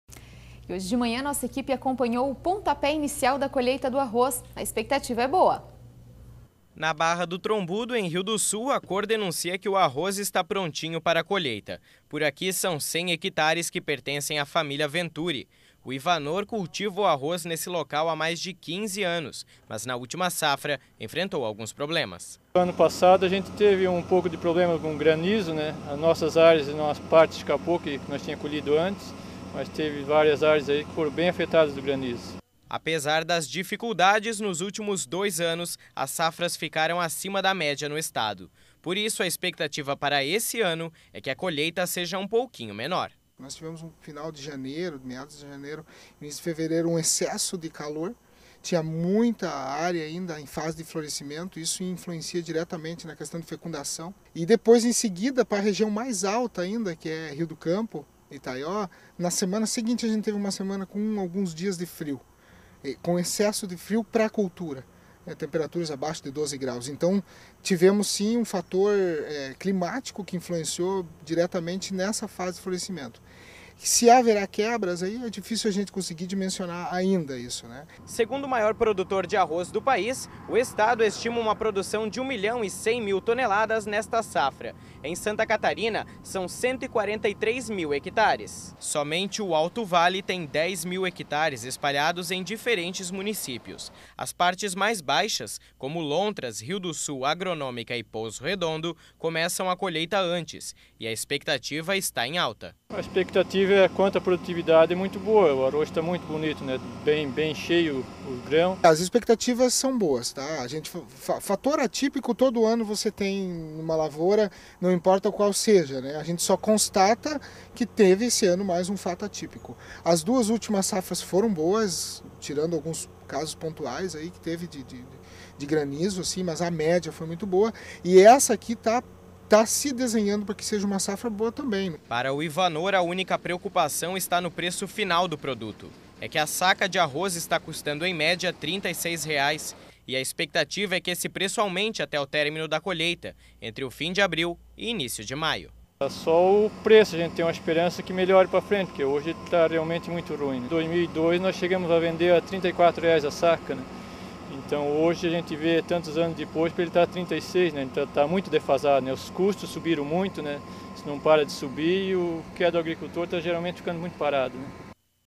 Começa a colheita do arroz
Colheita do Arroz
Nós acompanhamos o início da colheita do arroz em Rio do Sul. A expectativa é que a safra seja menor este ano em Santa Catarina, mas os produtores estão otimistas.